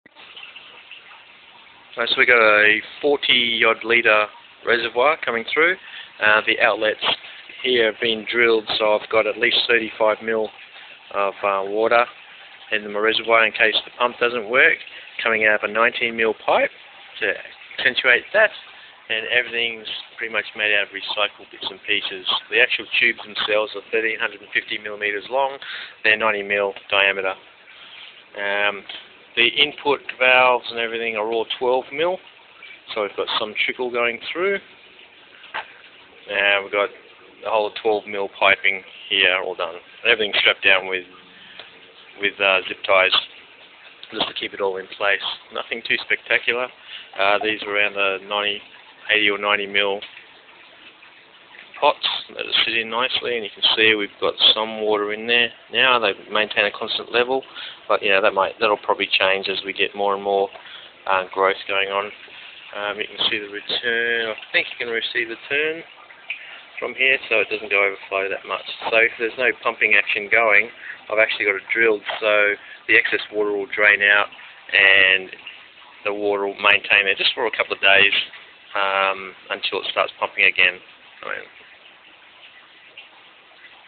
solar powered hydroponic specifications
The basic specs of my solar powered hydroponic system. I have been running it for 12 months now.